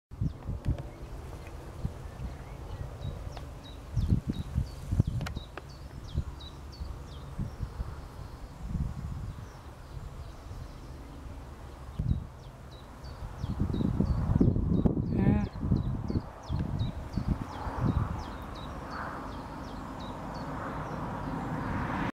Marbled White Butterflies
A group of marbled whites on a roadside verge near Ettington, Warwickshire, UK in July 2012